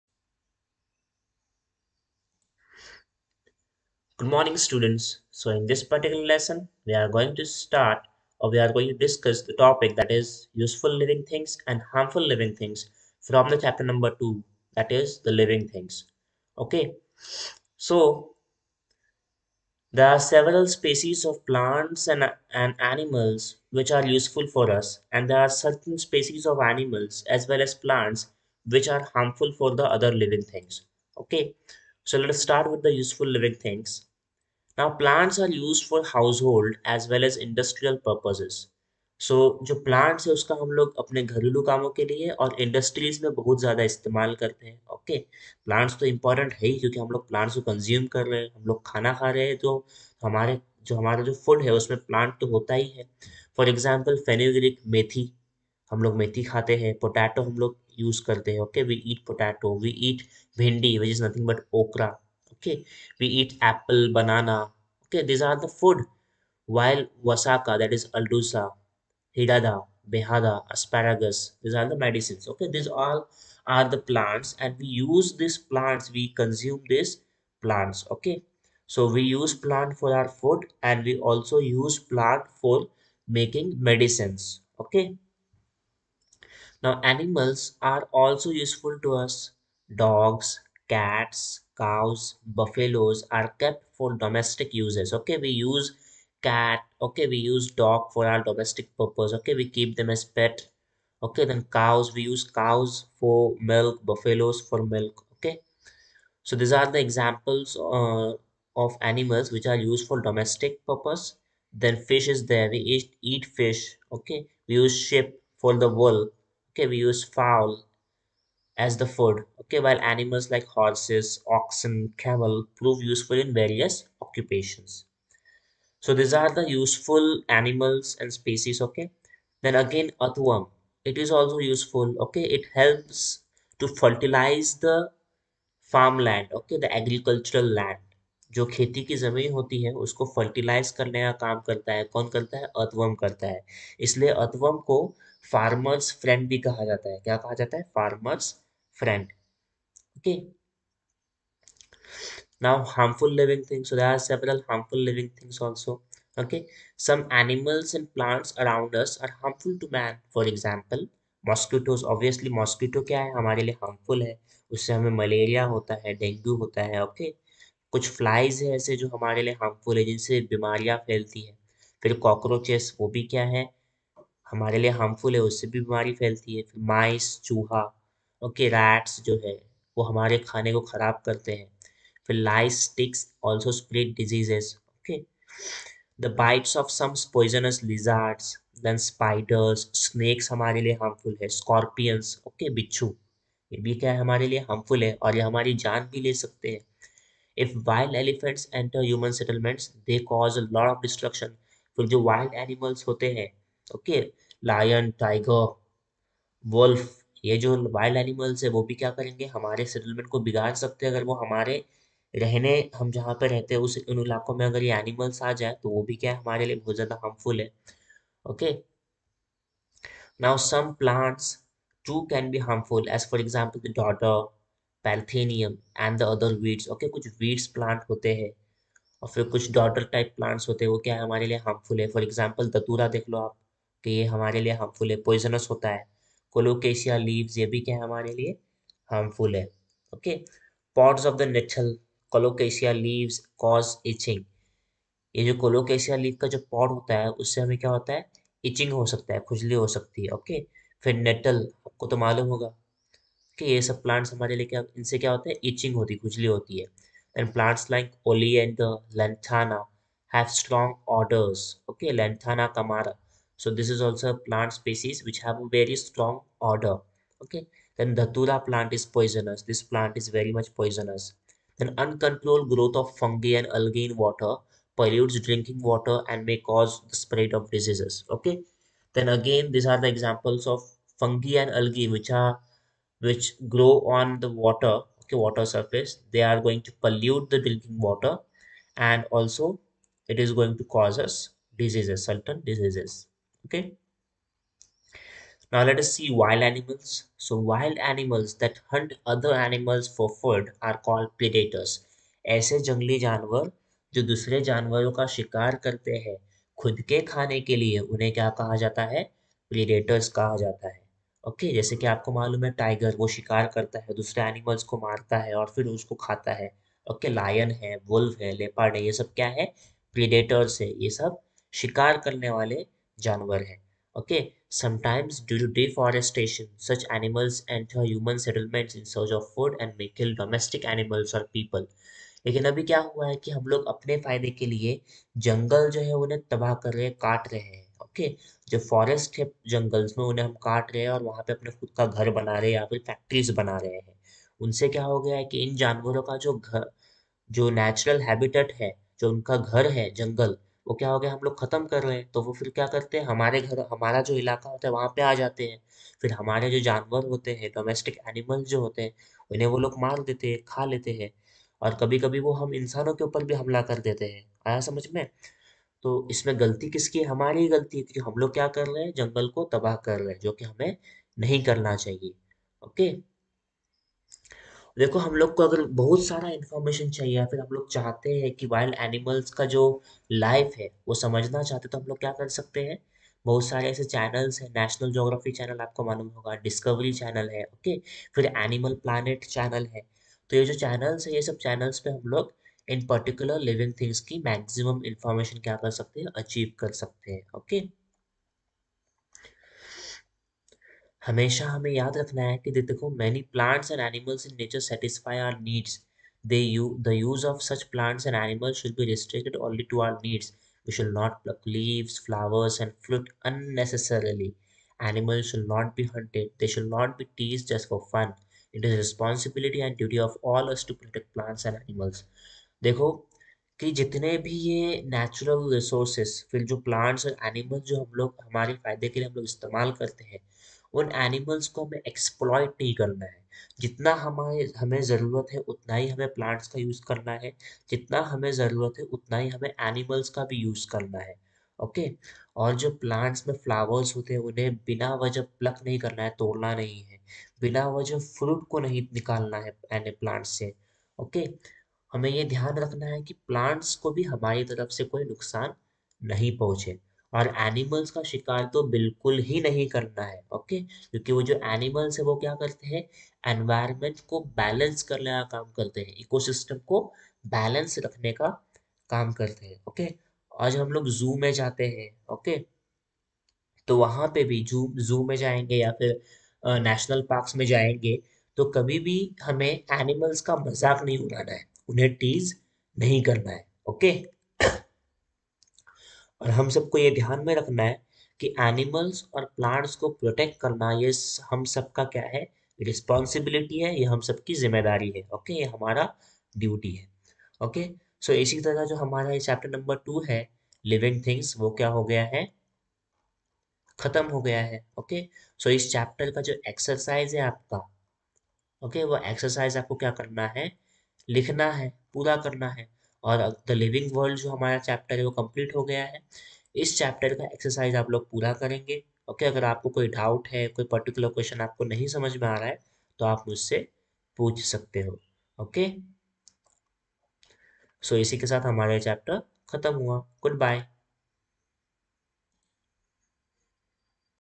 LEC 14 | CLASS 06 SCIENCE | THE LIVING WORLD
Welcome to our new lecture.
We are learning chapter no. 02 THE LIVING WORLD

Topics covered :
Useful and Harmful Living Organisms
Wild Animals

Homework : Complete Exercise Of The Chapter.

Stay Safe !